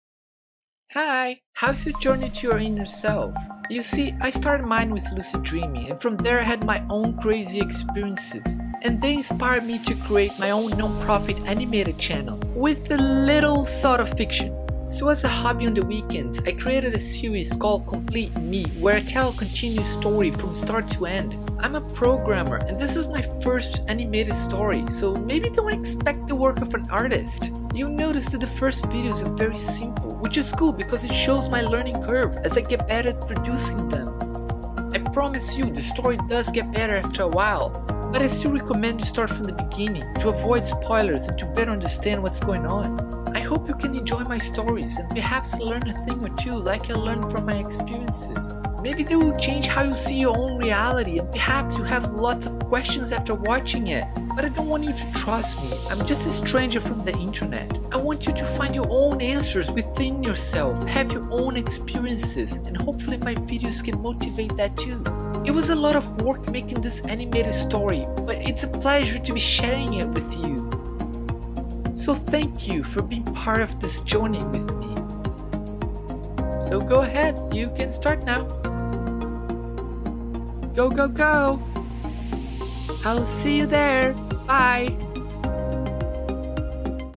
Intro to the lucid dreaming series
"Complete Me" is an animated story narrating the adventures of a kid in unusual lucid-dreaming experiences. It is a psychological journey to within the mind that leads the narrator to find life meanings that can drastically change his waking life.